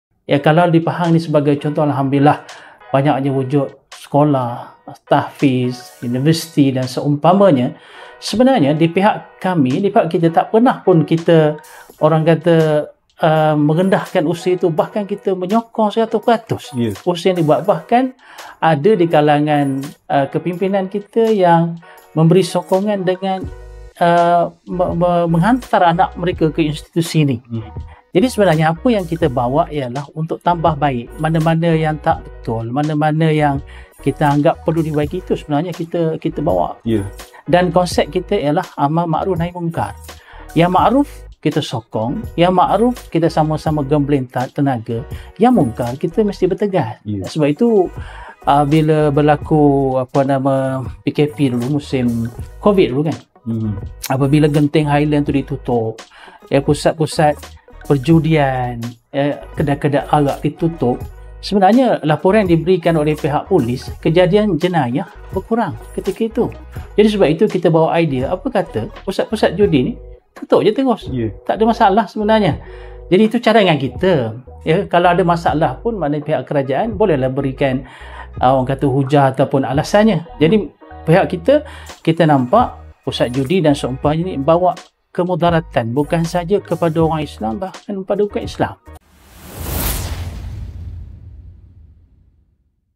Podcast PTP | PAS Menitikberatkan Amar Makruf Nahi Mungkar & Menyokong Usaha Penambahbaikan
Ulasan oleh Ahli Dewan Negeri Damak, YB. Ust. Zuridan Daud semasa siaran langsung Podcast PTP.

Nota:
Bagi yang ingin menyumbang untuk kos pengurusan & operasi  bulanan TV Makmur , boleh salurkan sumbangan anda ke dalam akaun di bawah.